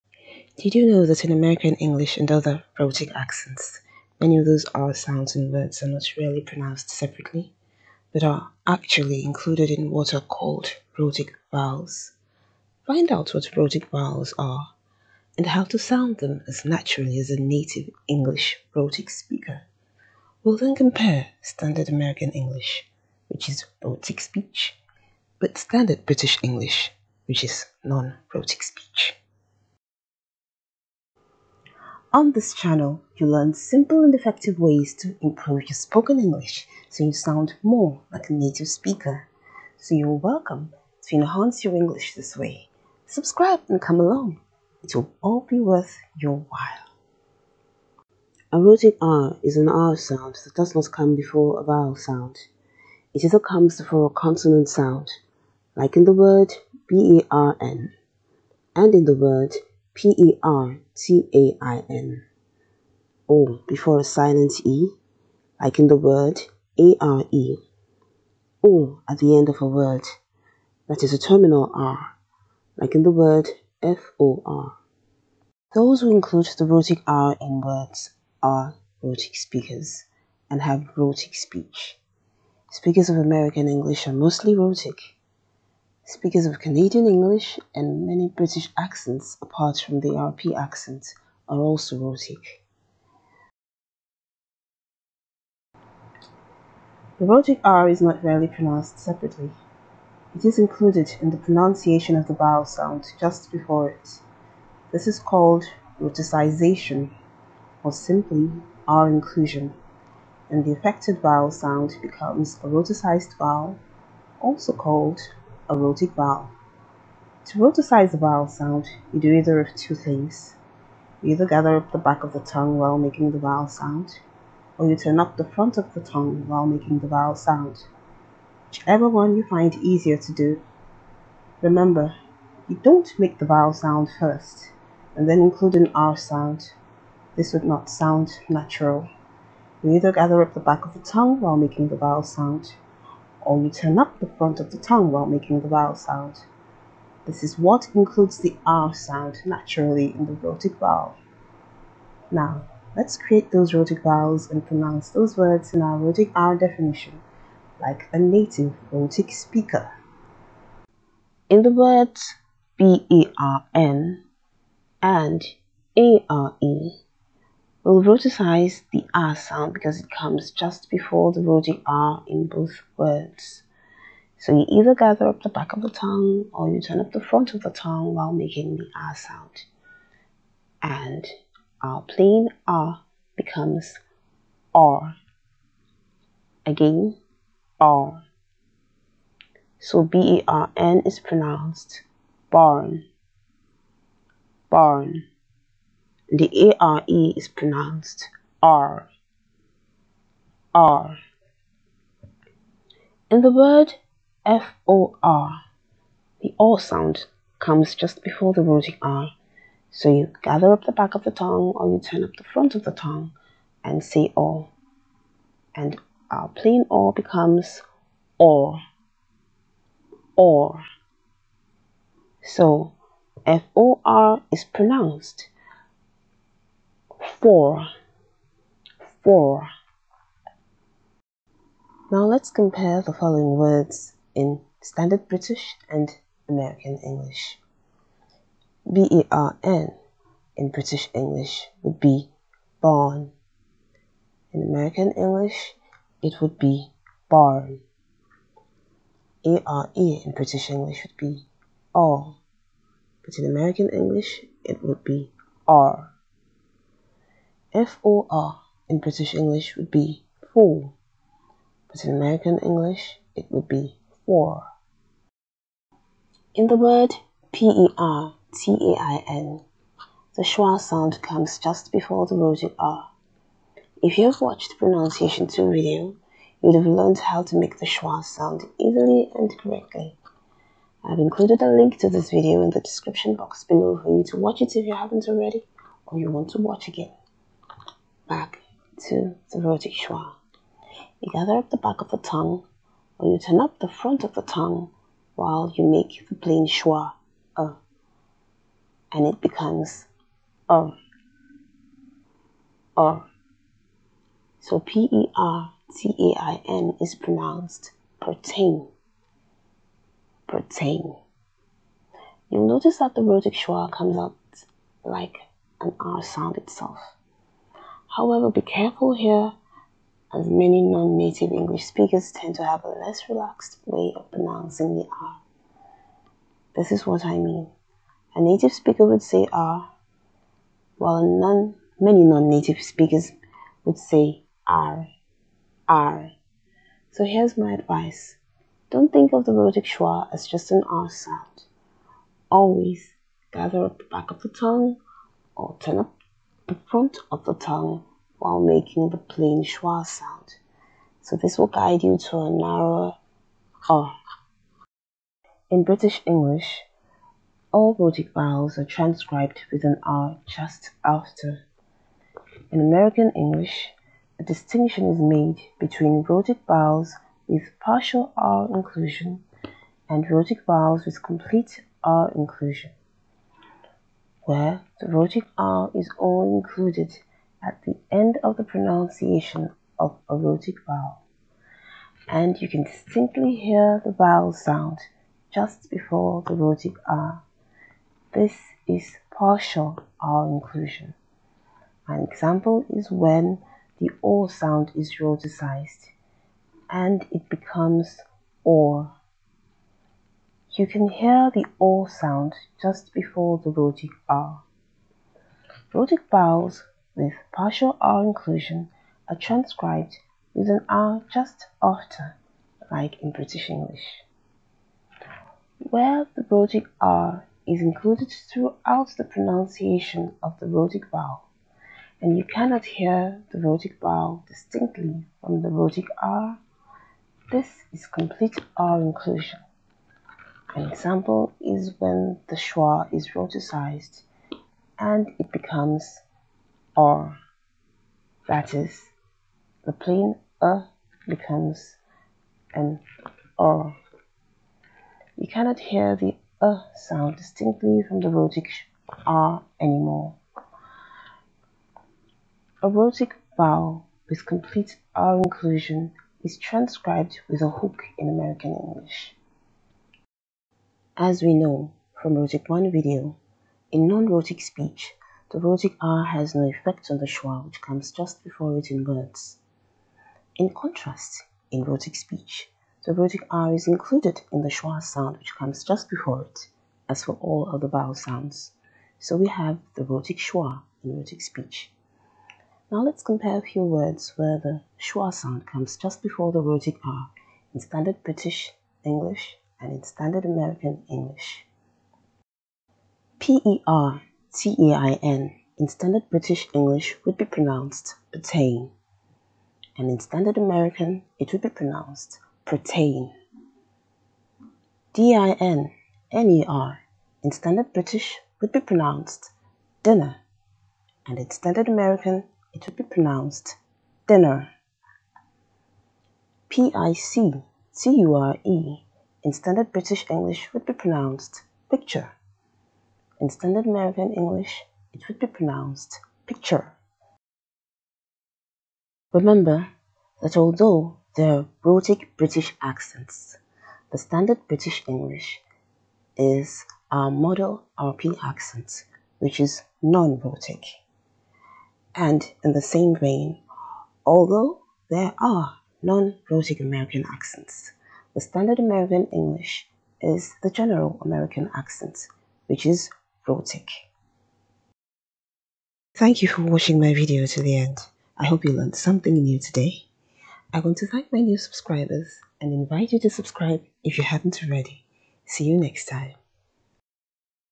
Pronunciation 4 – Rhotic R 2
Did you know that in American English and other rhotic accents, many of those ‘r’ sounds in words are not really pronounced separately, but are actually included in what are called rhotic vowels? Find out what rhotic vowels are and how to sound them as naturally as a native English rhotic speaker. We’ll then compare standard American English (rhotic speech) with standard British English (non-rhotic speech).